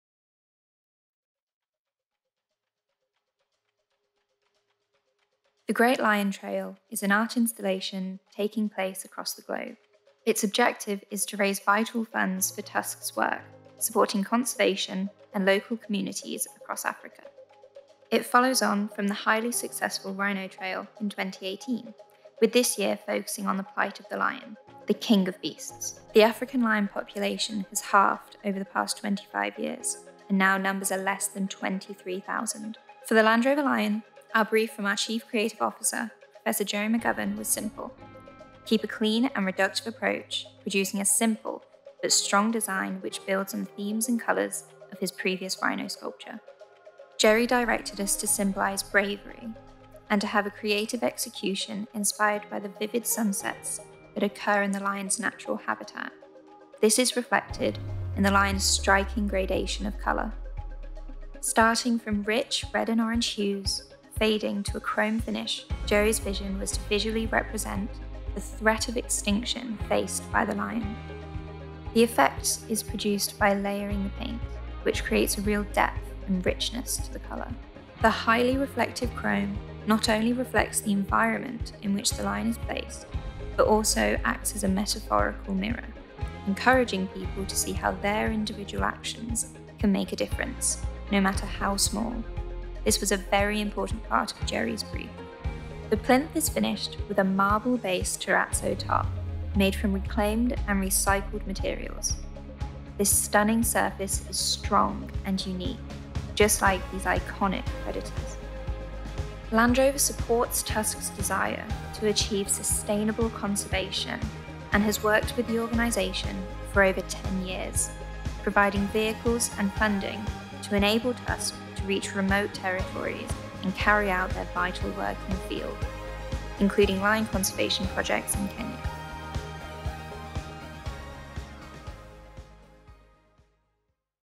Land Rover en World Lion Day 2021 | The Tusk Lion Trail
Op WorldLionDay werkt Land Rover mee aan de Tusk Lion Trail, een mondiale kunstinstallatie ter ondersteuning van natuurbescherming en afnemende leeuwenpopulatie in Afrika.